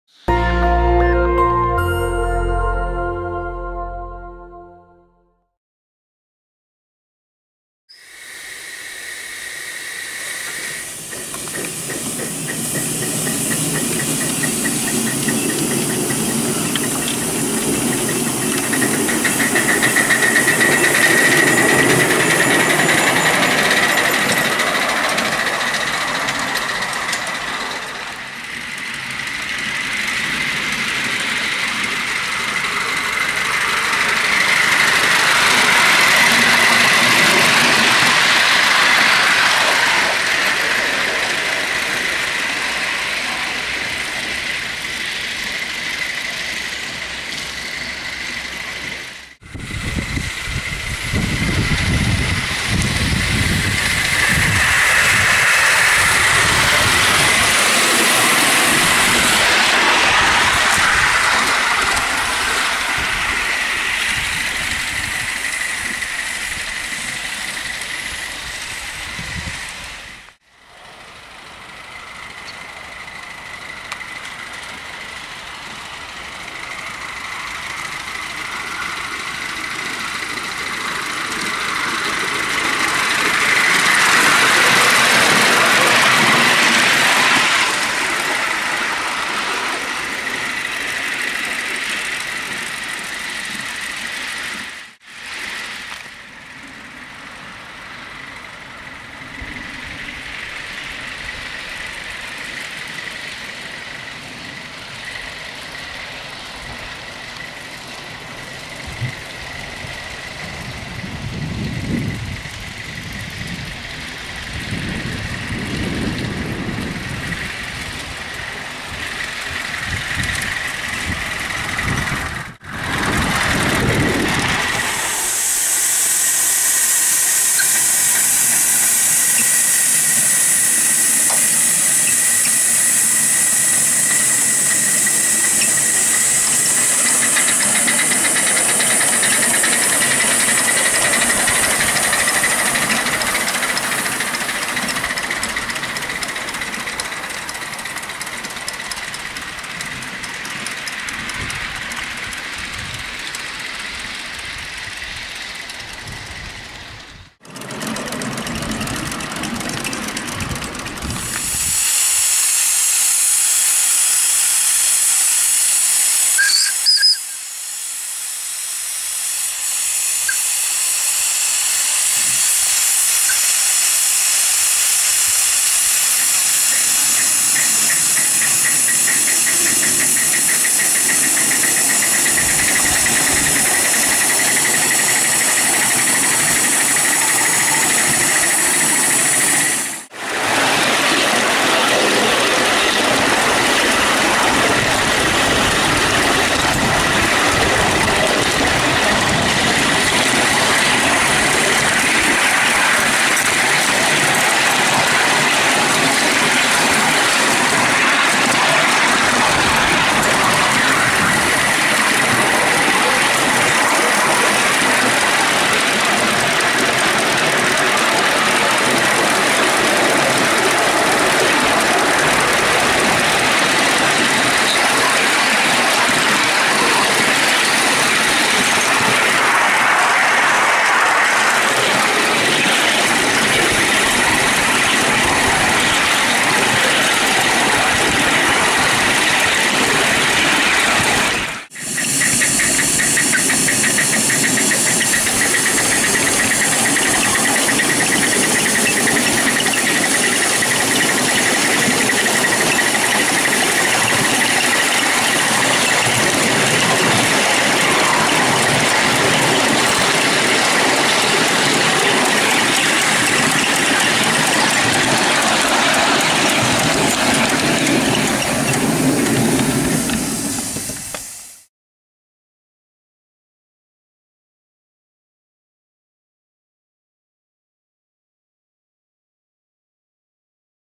5" Gauge 61XX Class Live Steam Locomotive
Introducing the 5 inch gauge GWR 61XX to our growing range of live steam locomotives. This 5" gauge model offers unbeatable value-for-money. The model is coal-fired and provides a powerful locomotive capable of pulling a number of adults.

The model is coal-fired and provides a powerful locomotive capable of pulling a number of adults. The model is delivered ready-to-run and fully painted, with a choice of either GWR Green, BR Lined Green or BR Lined Black livery. Each is complete with a silver soldered copper boiler, hydraulically tested to twice working pressure.

All boilers comply with the latest regulations and are appropriately marked and certificated. The 61xx makes an ideal model to display, transport and drive.

As testament to our confidence in the high quality of this model we are pleased to offer a full 12 month warranty. Our customer service is considered to be second-to-none.